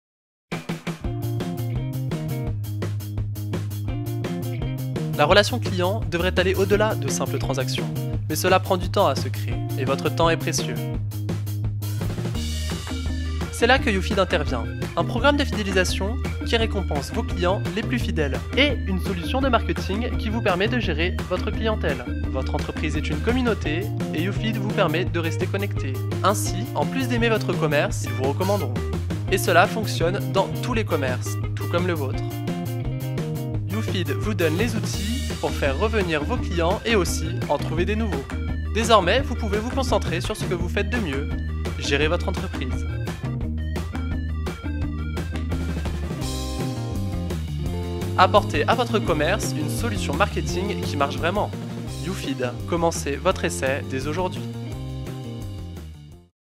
You Fid Application de fidélisation
You Fid est une application de fidélité dématérialisée qui vous récompense lors de vos achats. 2ème à droite réalise la nouvelle vidéo de présentation de l'application.

Annonceur : You Fid
Produit : Application Web
Production : 2ème À droite
Direction artistique & Réalisation : Avih
Post-Production : Avih